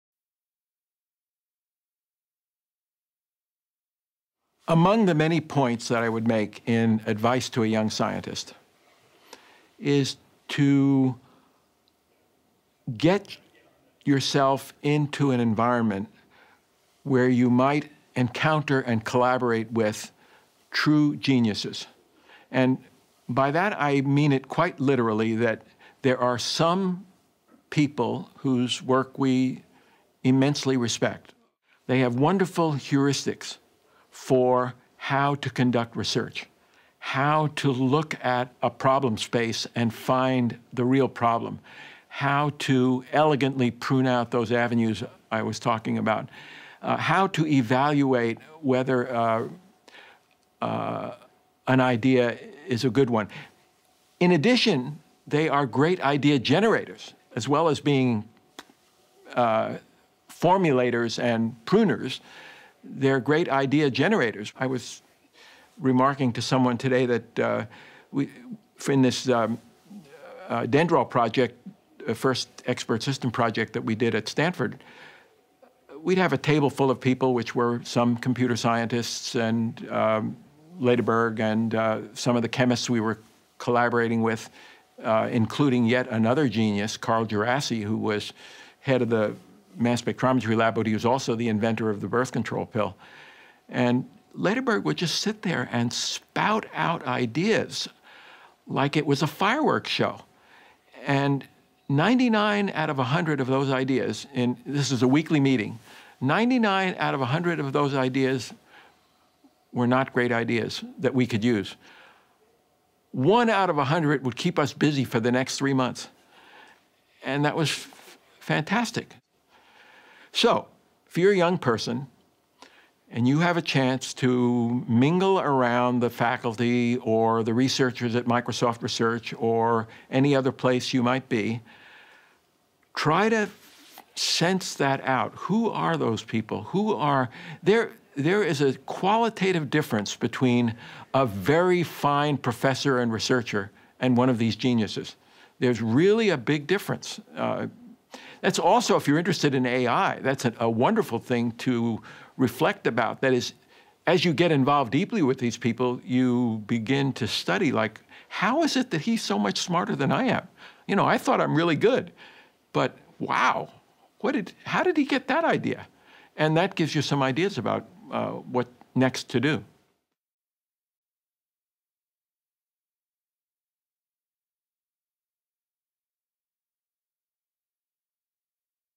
Surround yourself with geniuses
Six groups (teams Babbage, Boole, G├╢del, Turing, Shannon, and Simon), composed of Microsoft Research computer scientists and their colleagues from other research organizations, reflected separately about the past 20 years and the future to come, in the context of providing advice to young scientists. Afterwards, participants came together to listen to briefings about the discussions from the chairs of each team.     Edward Feigenbaum, Kumagai Professor of Computer Science Emeritus, Stanford University